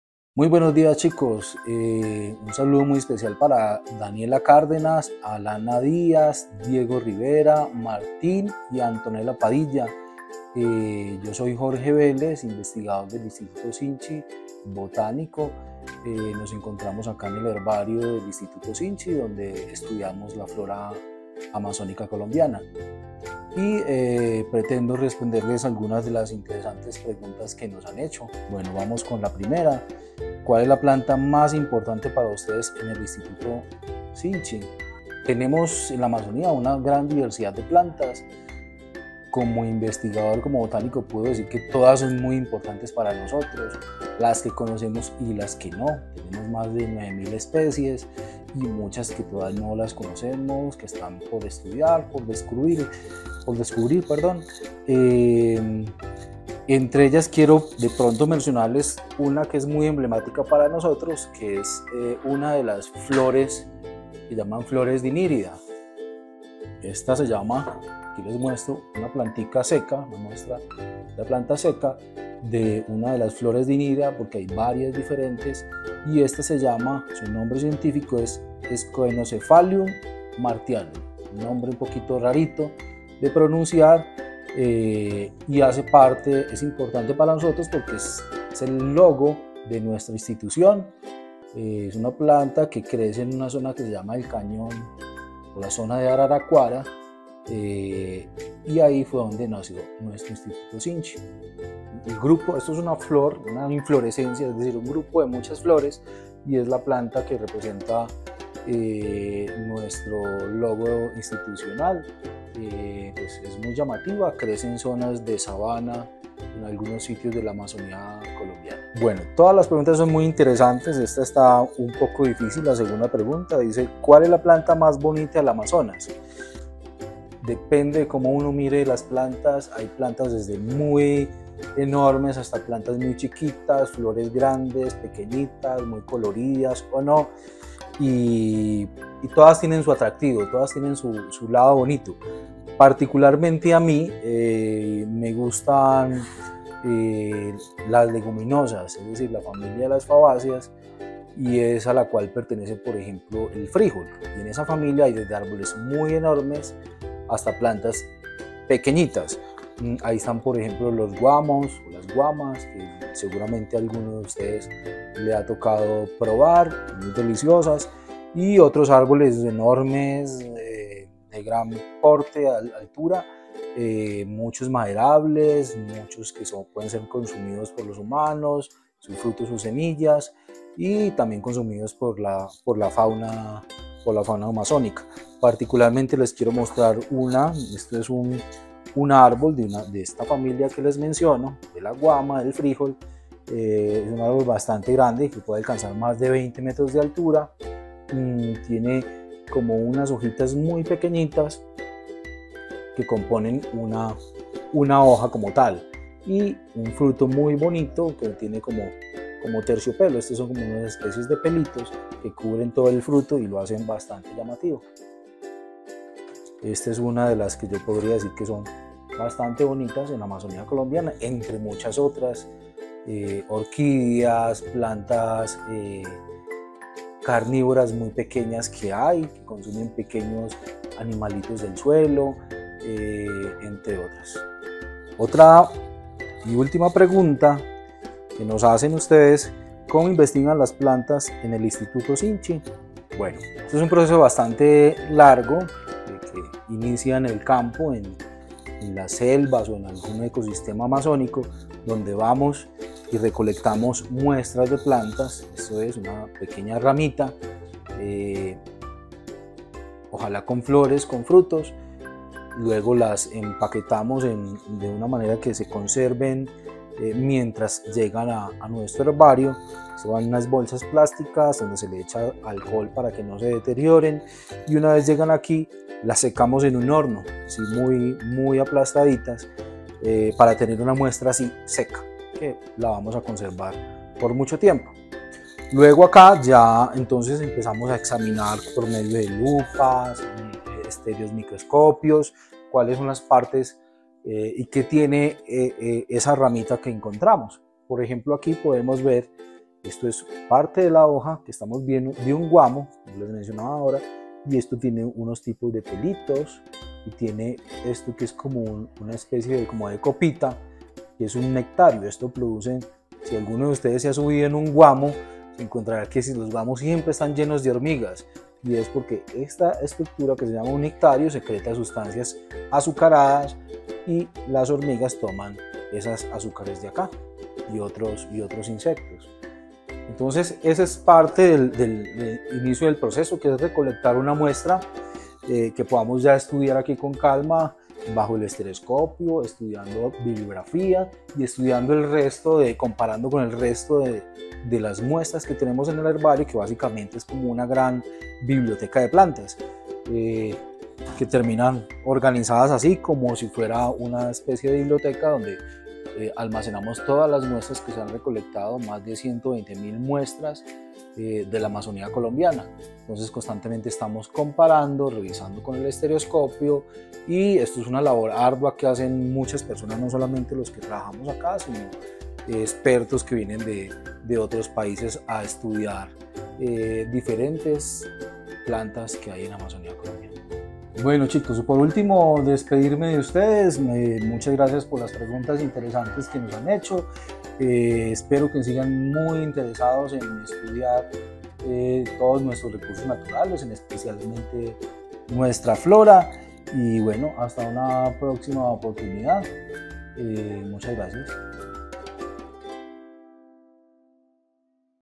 Instituto Amazónico de Investigaciones Científicas, SINCHI
¡Y llegaron las respuestas! 🙌🏻❓🤗

¿Recuerdas las cartas que nos enviaron los niños y niñas del Colegio Freinet de 📍Cali? ✍🏼📝

Tenemos la primera respuesta con el investigador Jorge Vélez. Mírala completa aquí⬆️